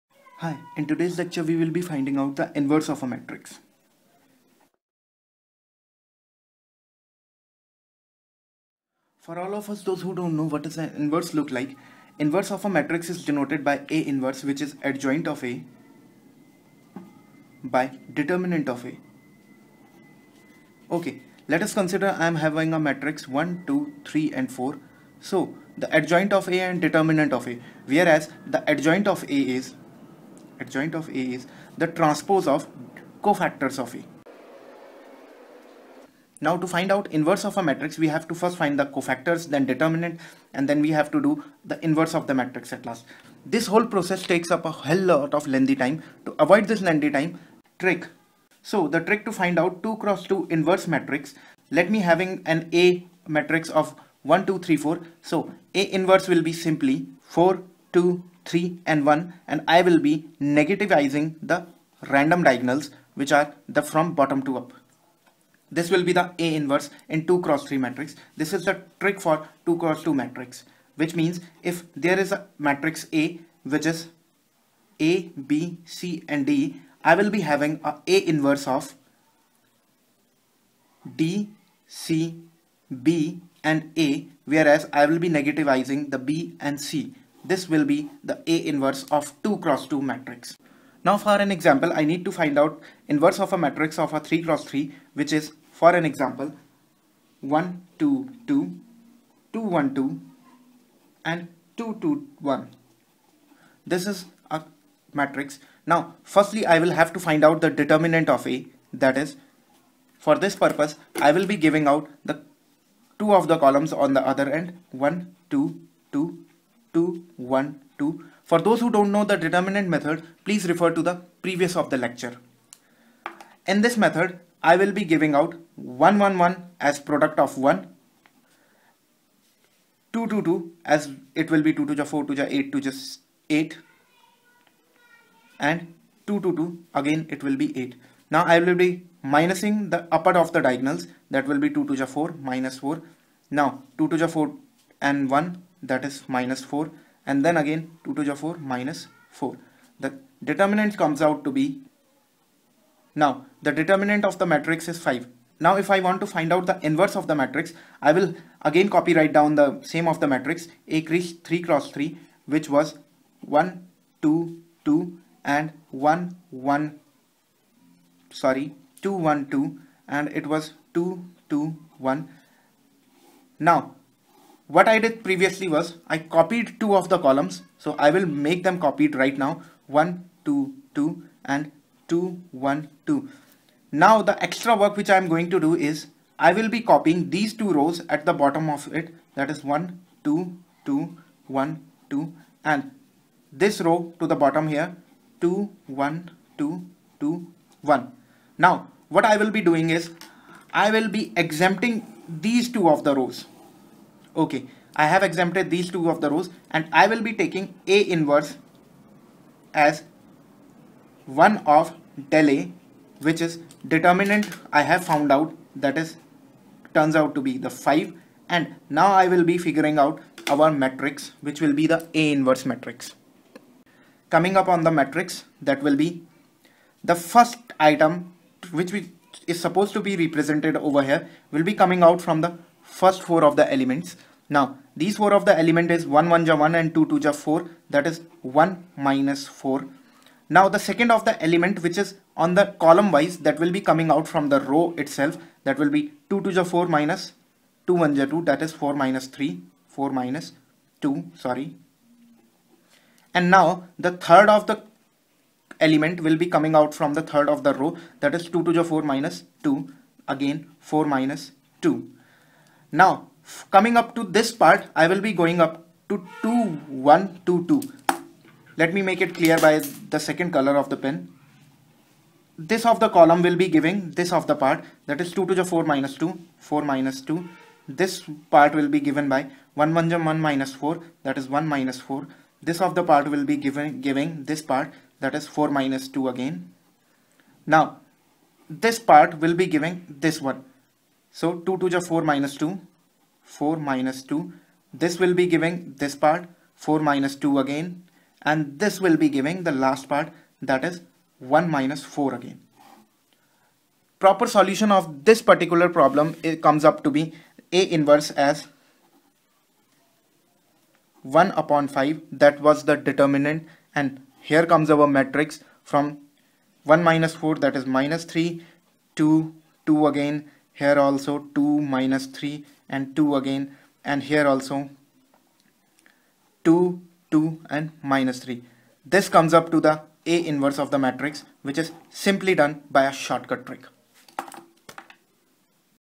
SHORT-CUT to INVERSE OF A MATRIX
Engineering Mathematics has a pare linear algebra in which matrices have inverse of a matrix. [A] matrix considered its inverse is 1/[A].
This lecture is to teach short cut trick of inverse of a matrix in Engineering Mathematics